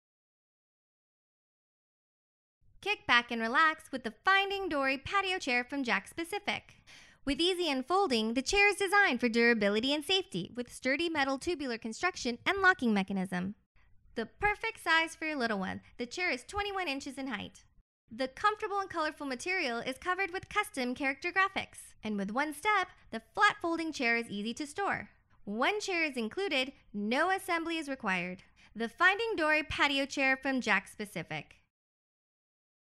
Finding Dory Patio Chair
Finding Dory Patio Chair - Item Number: 96861

Kick back and relax with the Finding Dory patio chair from Jakks Pacific. With easy unfolding, the chair is designed for durability and safety, with sturdy metal tubular construction and locking mechanism. The perfect size for your little one. The chair is 21 inches in height. The comfortable and colorful material is covered with custom character graphics. And with one step, the flat folding chair is easy to store. One chair is included. No assembly is required. The Finding Dory patio chair from Jakks Pacific.